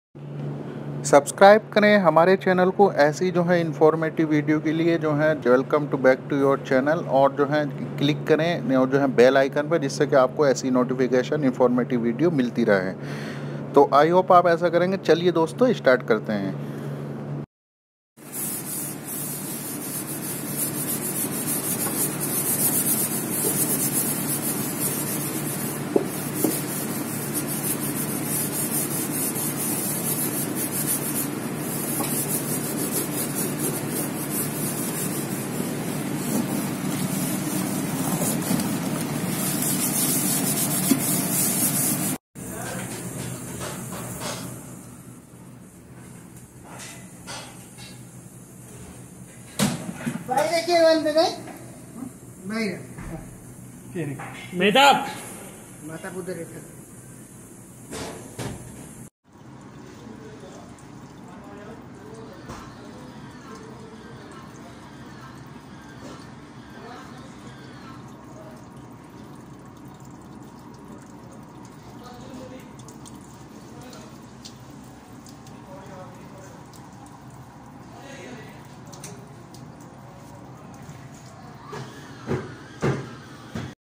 Kitchen Deep Cleaning Tips And Tricks ! Kitchen Clean Ideas ! Clean My Kitchen With Me 2020
This Is The Best Way To Get Clean Your Section In Kitchen Department

About This Video - IS VIDEO MAI AAP DEKHENGE KI KAISE KITCHEN MEIN AAP DEEP CLEAN KRWA SAKTE HAI

Topic Cover - kitchen department in hotel industry

kitchen stewarding department in hotel

kitchen stewarding training manual